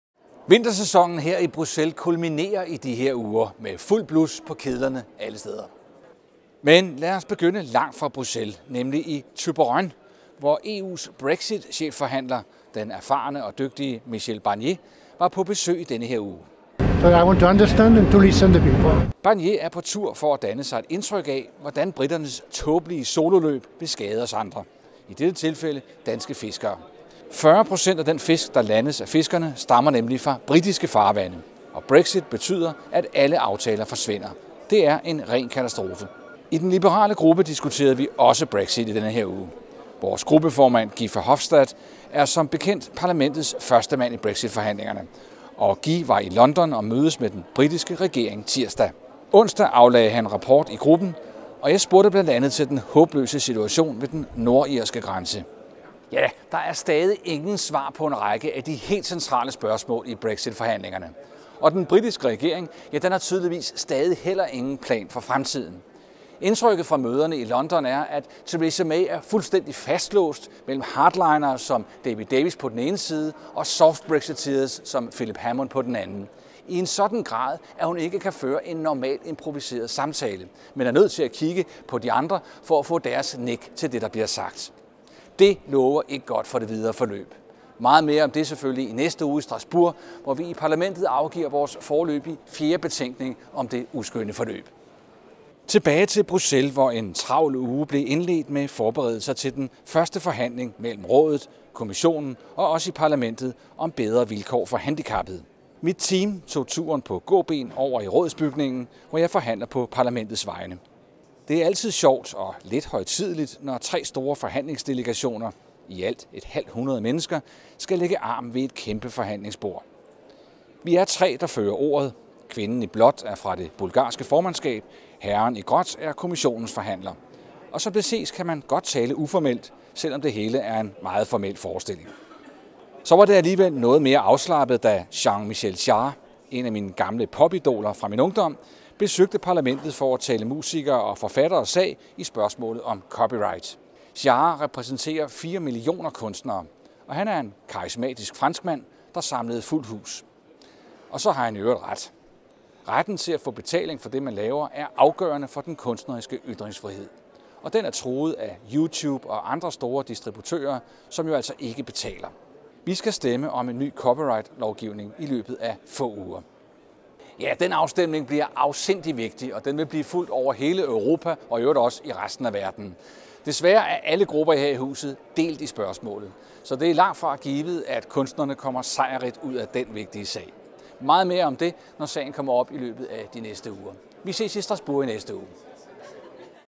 Morten Løkkegaard om Brexit-forhandlinger og Jean Michel Jarre
Har briterne en plan for fremtiden efter Brexit? Få svaret og andre insights i denne uges LøkkegaardTV